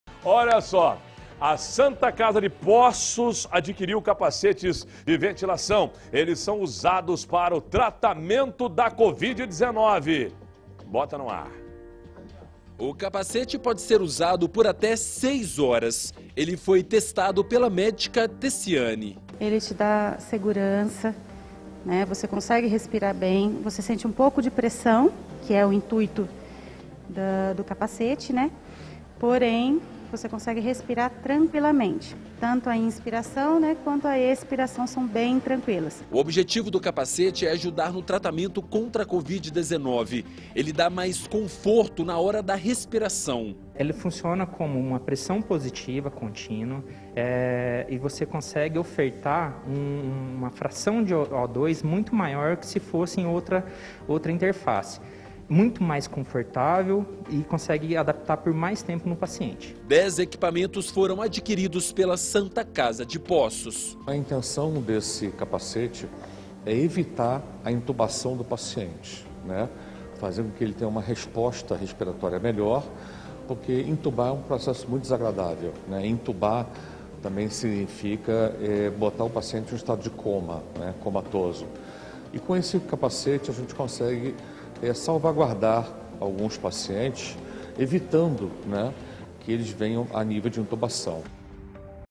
Capacetes de ventilação para a Santa Casa de Poços
CAPACETES DE VENTILAÇÃO PARA A SANTA CASA DE POÇOS | A Santa Casa de Poços de Caldas adquiriu capacetes de ventilação, eles são usados para tratamento da covid-19.